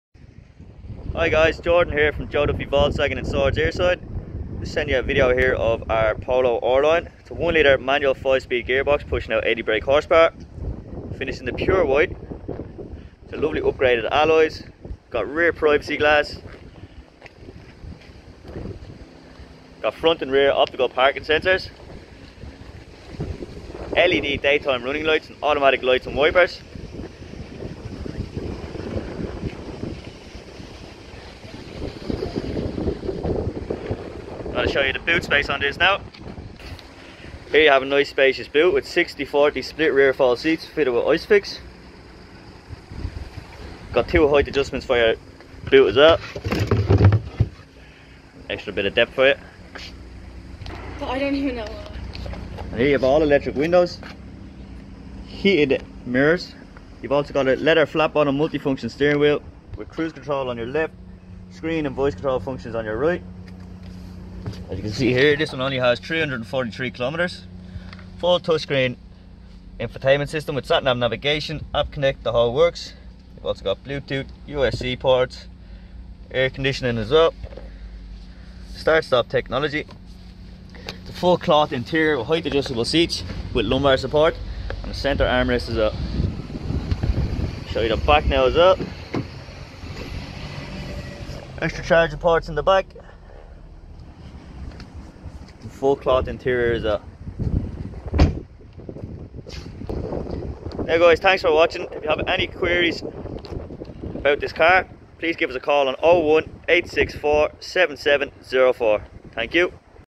212D8769 - 2021 Volkswagen Polo RLINE M5F 80HP 25,900
Here is a video of our 212D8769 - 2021 Volkswagen Polo RLINE M5F 80HP 25,900. Please contact SALESMAN NAME to arrange a test drive today. Joe Duffy Volkswagen Swords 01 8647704 infojoeduffyvolkswagen.ie